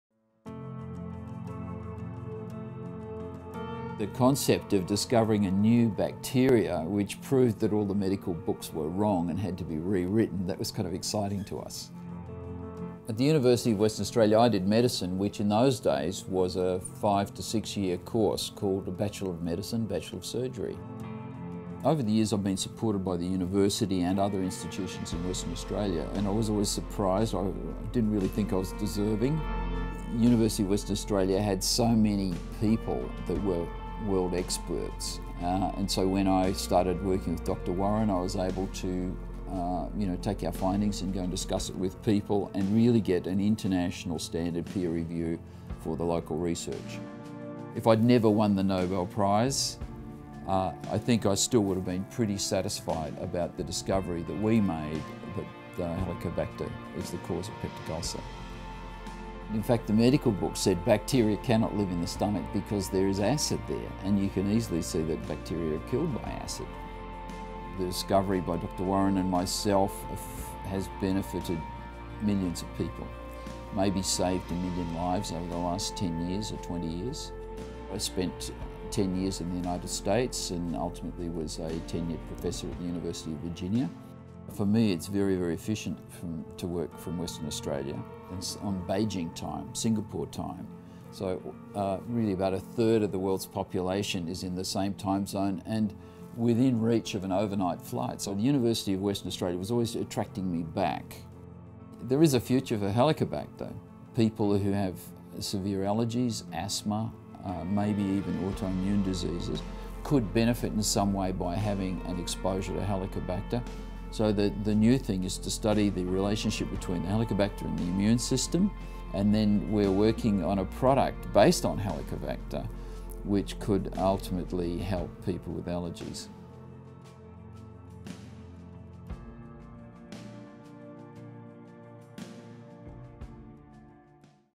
Professor Barry Marshall - Nobel Laureate
Barry Marshall talks about bacteria